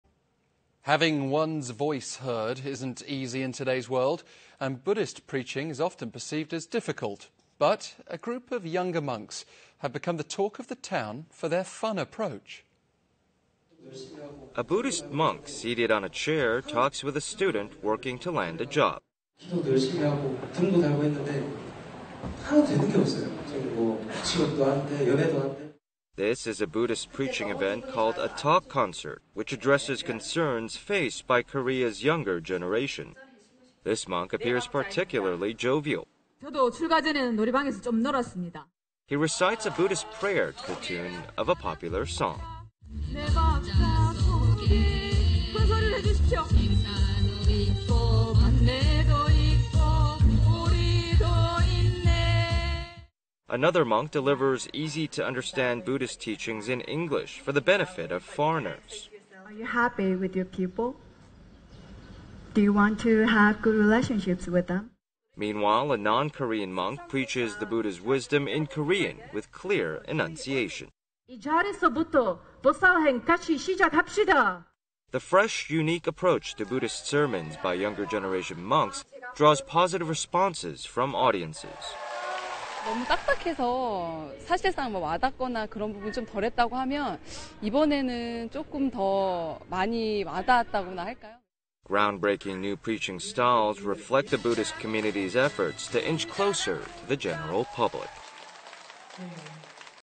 Pop Buddhism
[Anchor Lead]

Having one’s voice heard isn’t easy in today’s world, and Buddhist preaching is often perceived as difficult. But a group of younger monks have become the talk of the town for their fun approach.

[Pkg]

A Buddhist monk seated on a chair talks with a student working to land a job.

[Soundbite] "I prayed hard and even lit a lantern, but nothing has worked out in job-seeking or dating."

This is a Buddhist preaching event called a "talk concert" which addresses concerns faced by Korea's younger generation. This monk appears particularly jovial.

[Soundbite] Ven. Myeongjeong : "Before entering priesthood, I also had a lot of fun singing."

He recites a Buddhist prayer to the tune of a popular song.

[Soundbite] "In the four beats, there is life and death, anguish and wisdom~♬"

Another monk delivers easy to understand Buddhist teachings in English, for the benefit of foreigners.

[Soundbite] Ven. Seongyeong

Meanwhile a non-Korean monk preaches the Buddha's wisdom in Korean with clear enunciation.

[Soundbite] Ven. Hyujeong : "Let us train together from here onward!"

The fresh, unique approach to Buddhist sermons by younger generation monks draws positive responses from audiences.

[Soundbite] Lee Jeong-ja : "The preaching was too formal and less relatable. But today, I felt more connected."

Groundbreaking new preaching styles reflect the Buddhist community's efforts to inch closer to the general public.